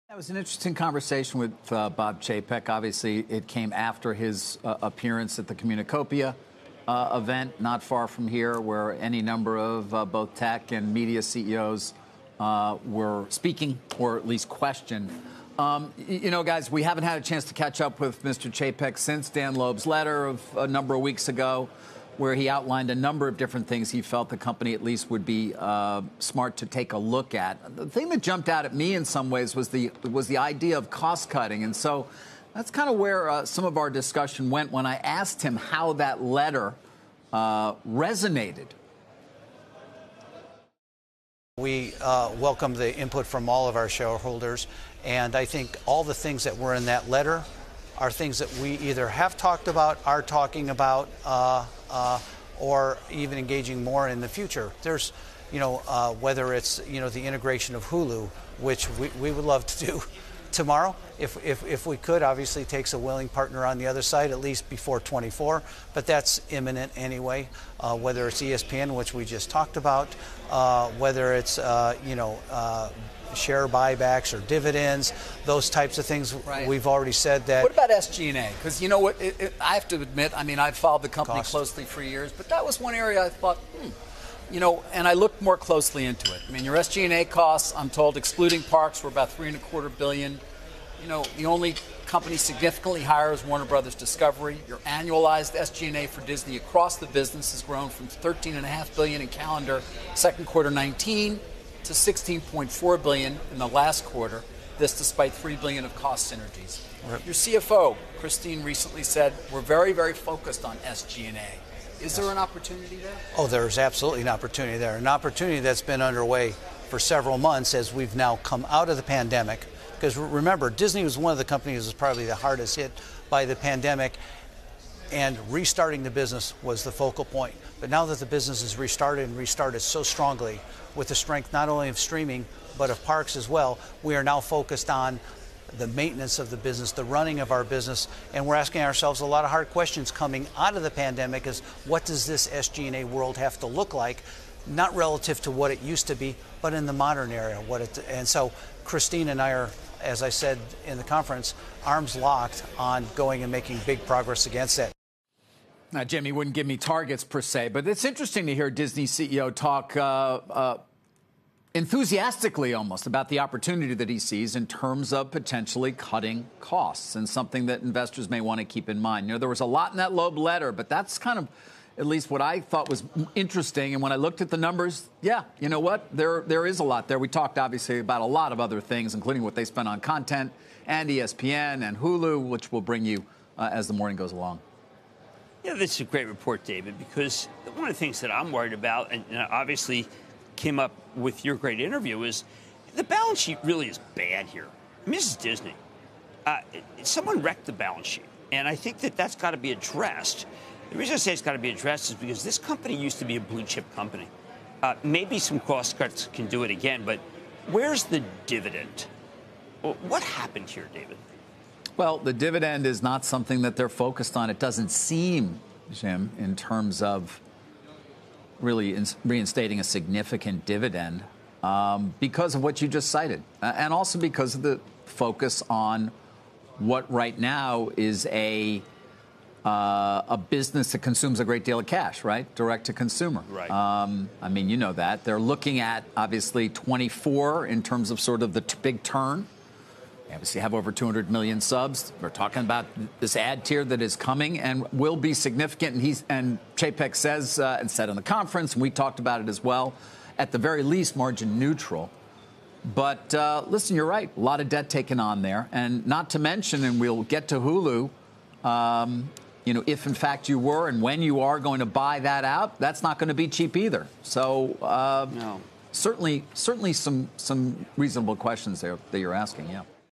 Disney CEO Bob Chapek on rising operating costs, activist investor Dan Loeb
CNBC's David Faber sat down with Disney CEO Bob Chapek to discuss the company's streaming strategy and its integration with Hulu amid pressure from activist investor Daniel Loeb.